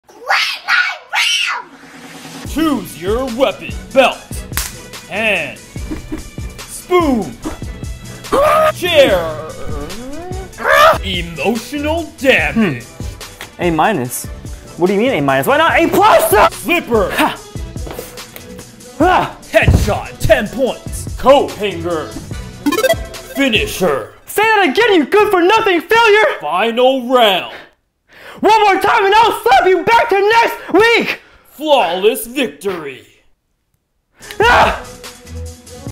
Asian Moms Be Like 😂
Hey Ian Nation! This video is Part 1 of What It's Like Growing Up Asian! Has this ever happened to you?